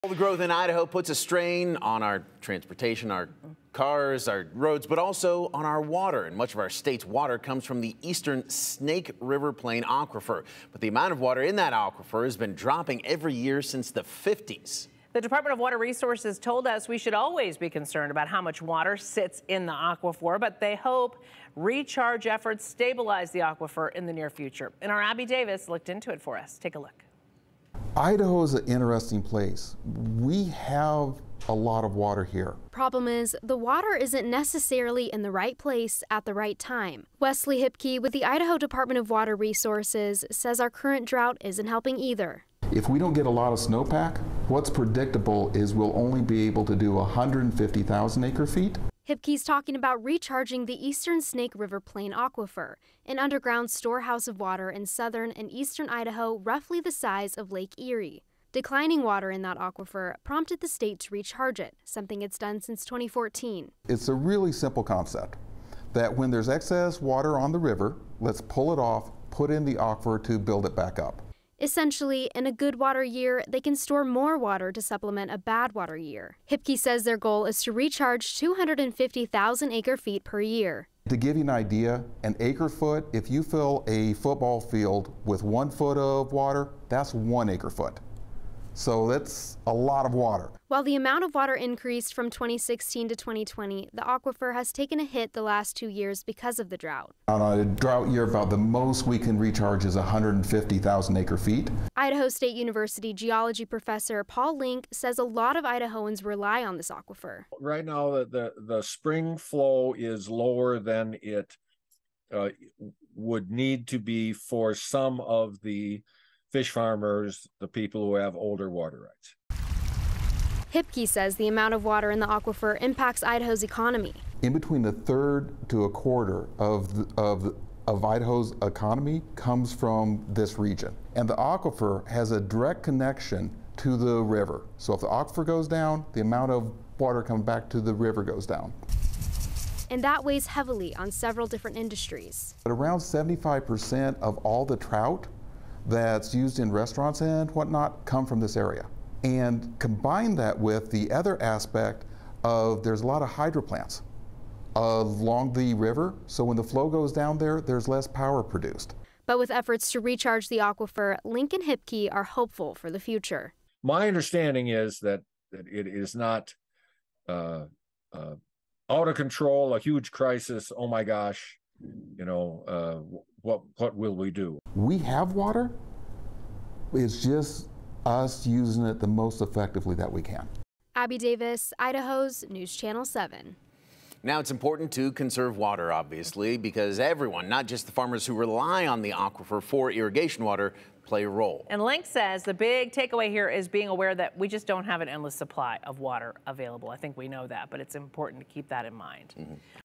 Eastern Snake River Plain Aquifer impacted by drought
In a good water year, more water is stored to supplement a bad water year – something that is more difficult during a drought.